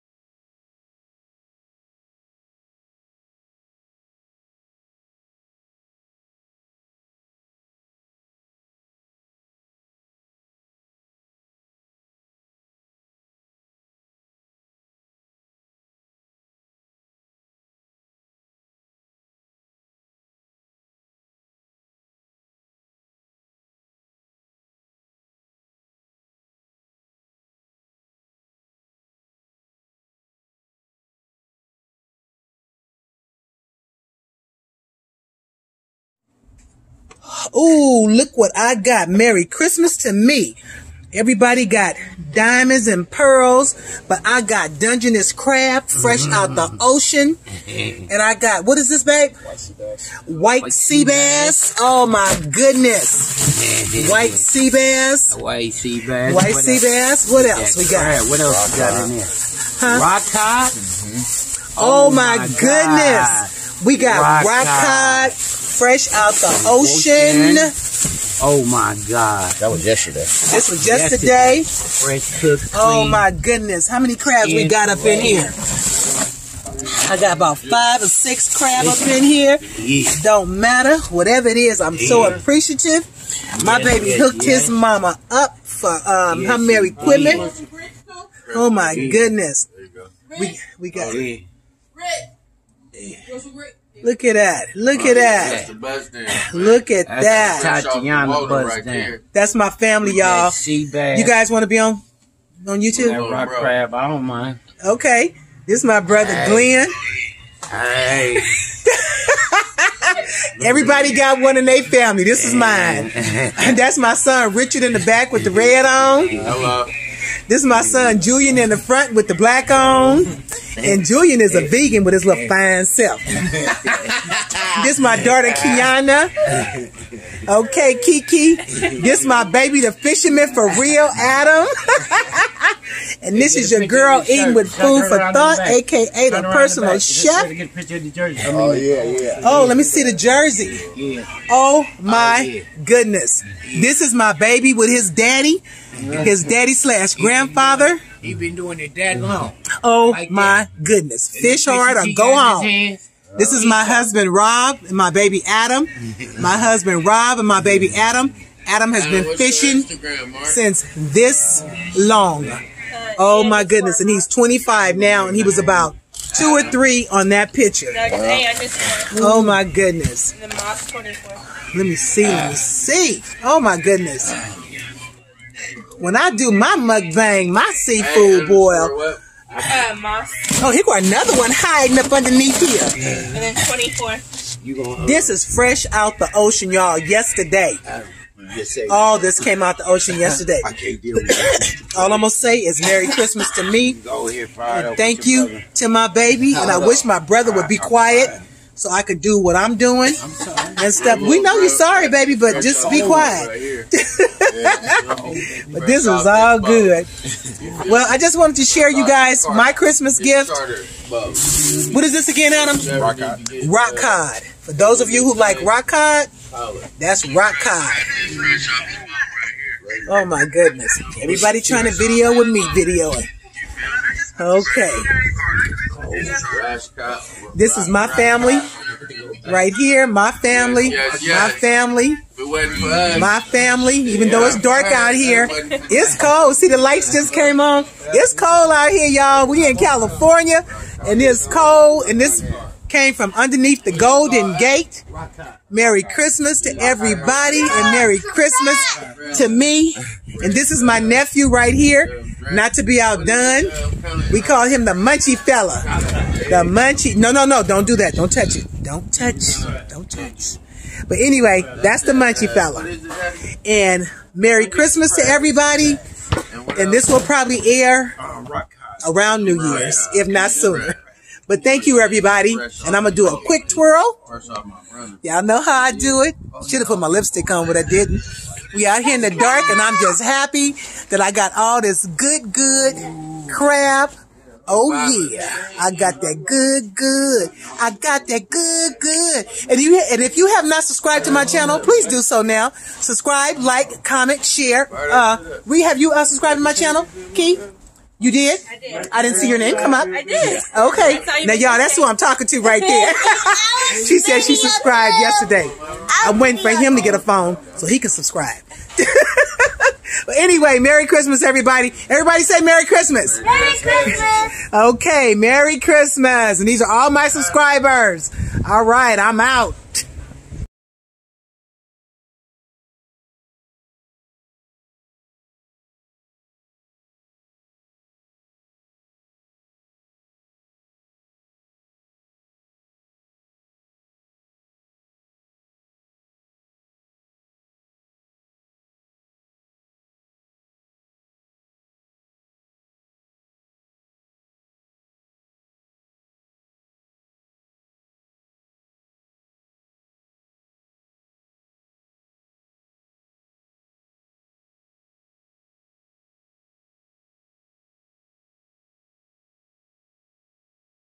Dungeness Crab
My Merry Christmas Present from my son Adam the Fisherman... Merry Christmas everyone!!
A short quick video.